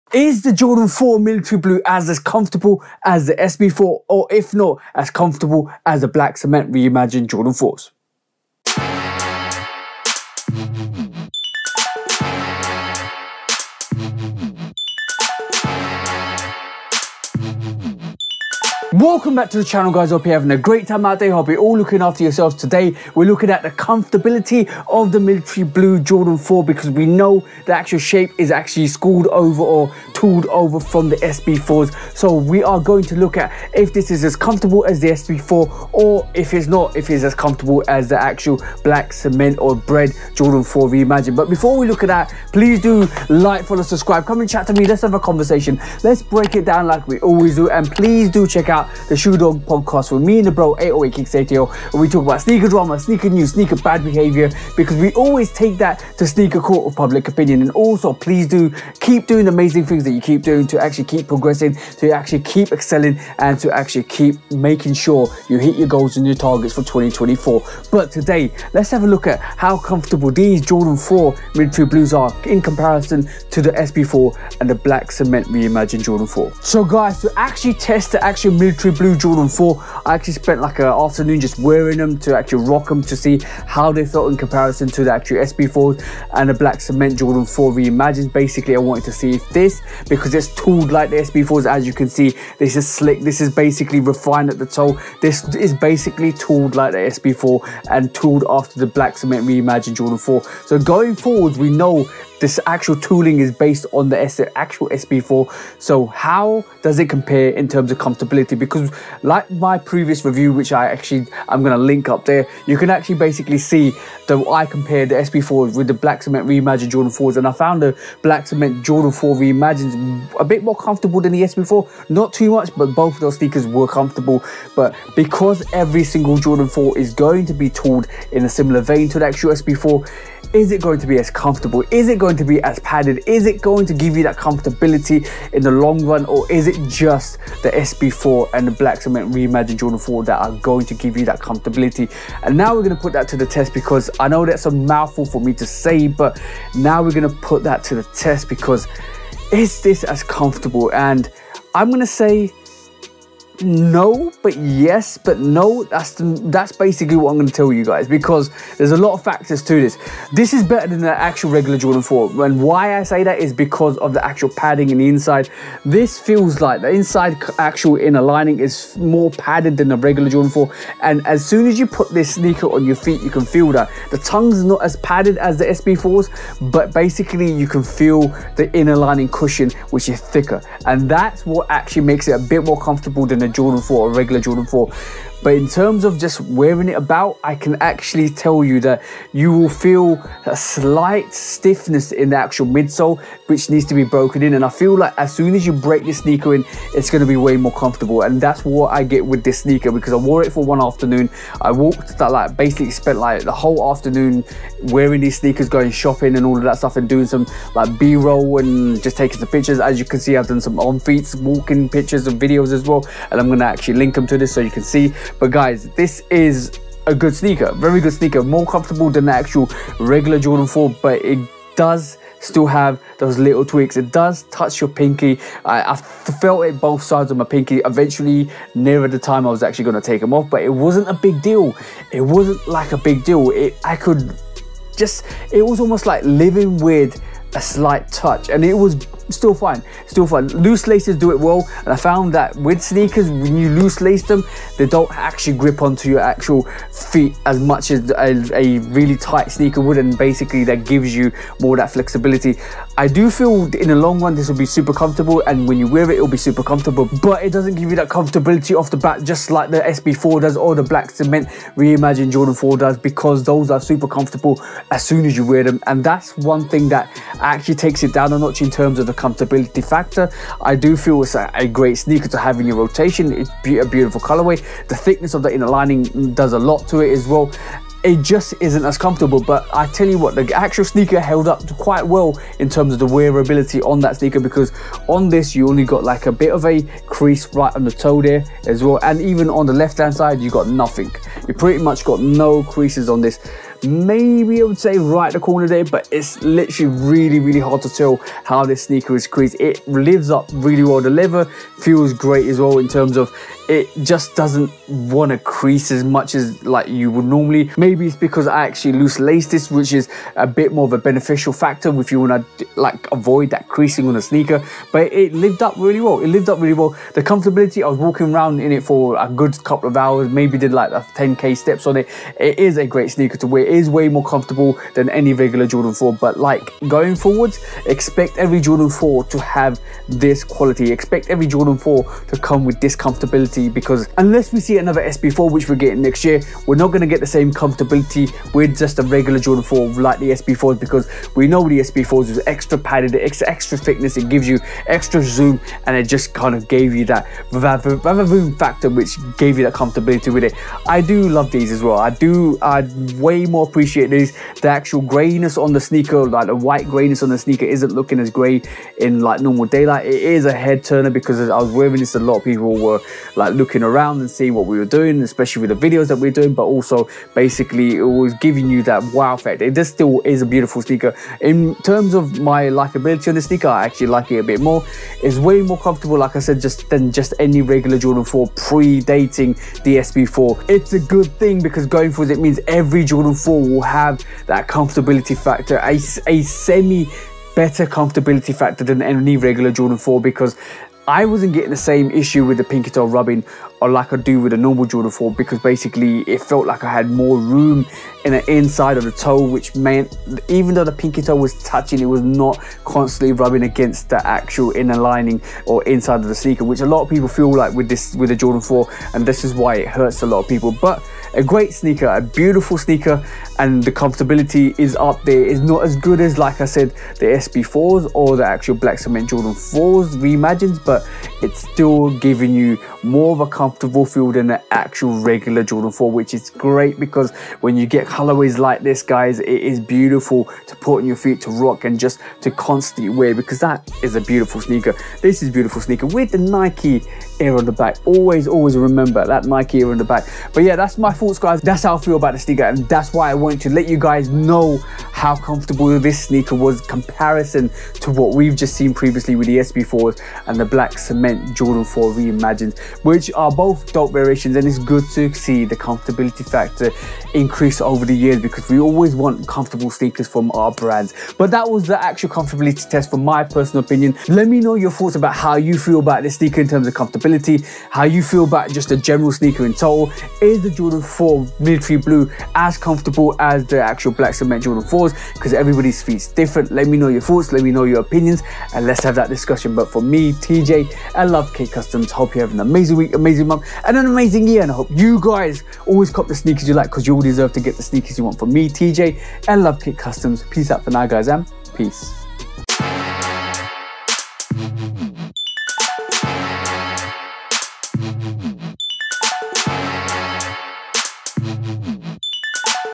MILITARY BLUE JORDAN 4, IS IT AS COMFORTABLE AS THE JORDAN 4 SB
The big question today is whether the Jordan 4 Military Blue is as comfortable as the Jordan 4 SB or the Jordan 4 Black Cement Re-imagined.

Well I put that to the test this week to see if the new shape and tooling replicates the comfort of the the Jordan 4 SB or Jordan 4 Black Cement Re-imagined.

Tune in and find out and let me know your personal thoughts.

Timecode

00:00 Intro
01:16 Comfort test

Have an amazing Week, amazing Month and have an amazing Year, and I hope you cope the sneaker you want because we all deserve to cop the sneakers we like.